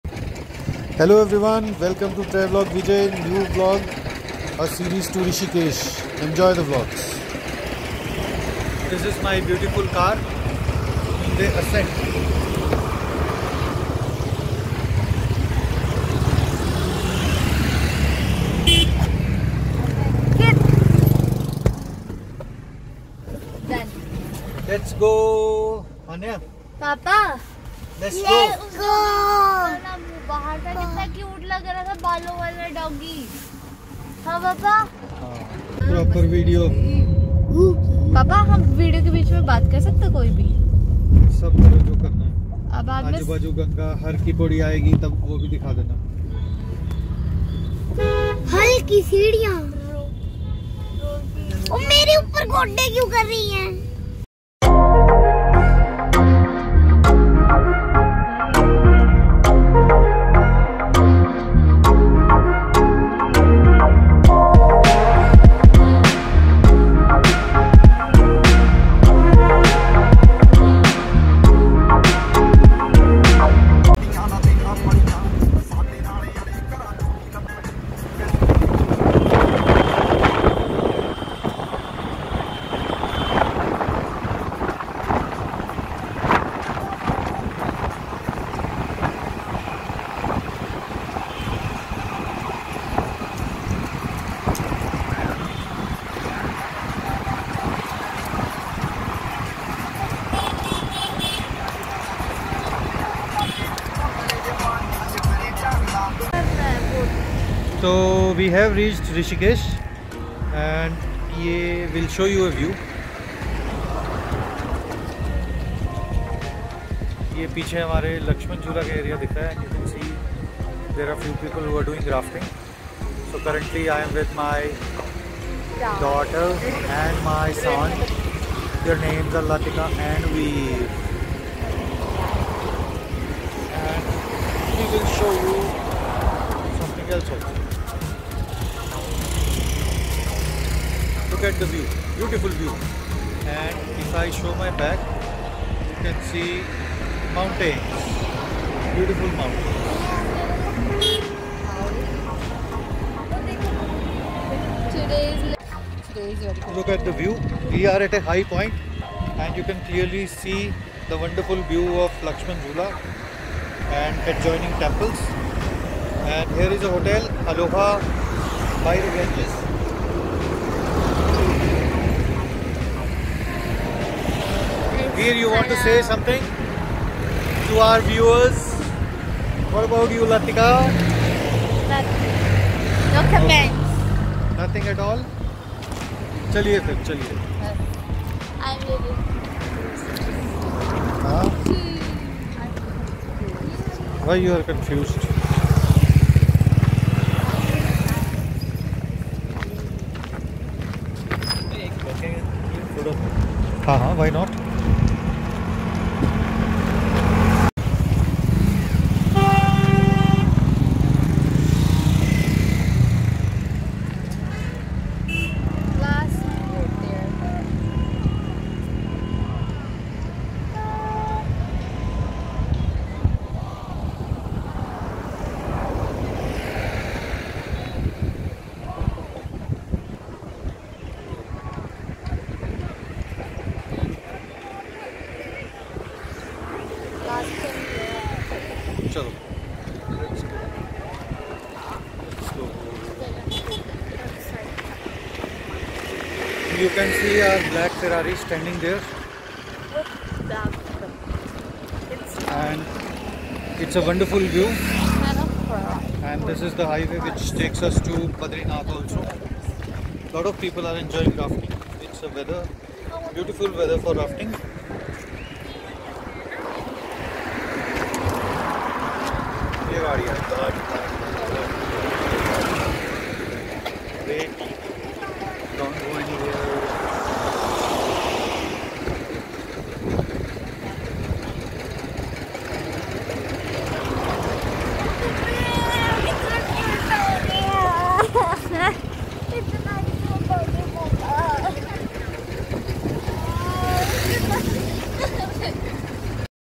trip with family Vlog1
Rishikesh Hillstation family trip .Ganga
This vlog covers my family trip with kids to rishikesh and back to haridwar. This will be series of vlogs specially on Rishikesh